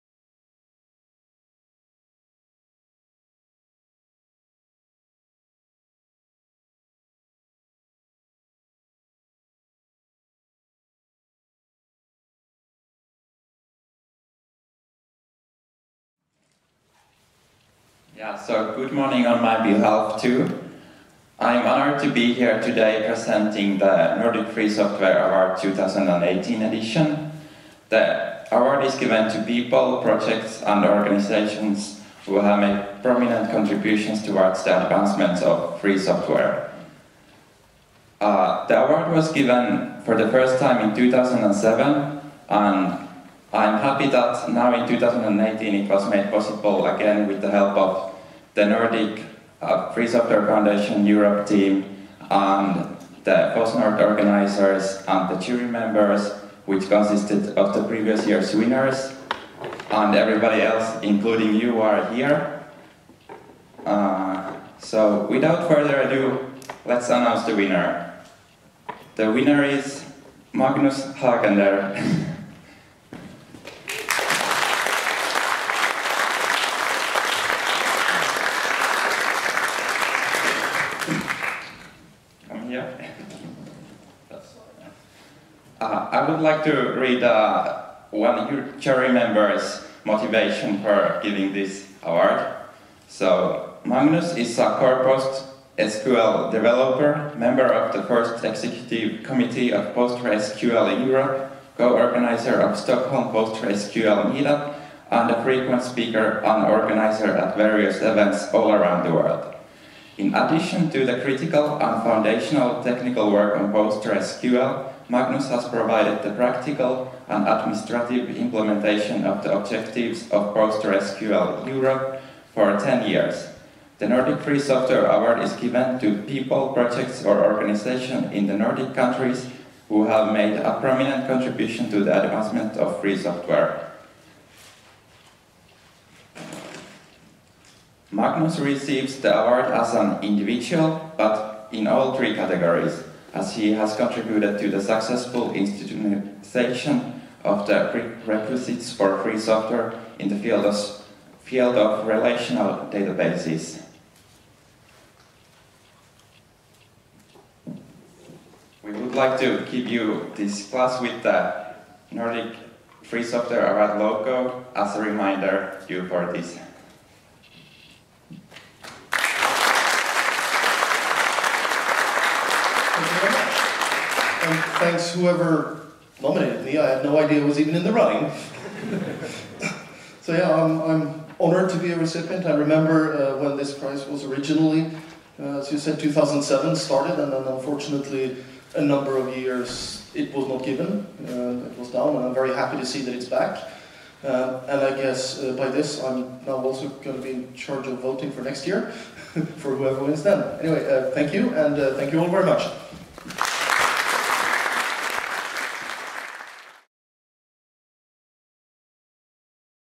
The Nordic Free Software Award Prize Ceremony 2018
The Nordic Free Software Award is given to people, projects or organisations in the nordic countries that have made a prominent contribution to the advancement of Free Software.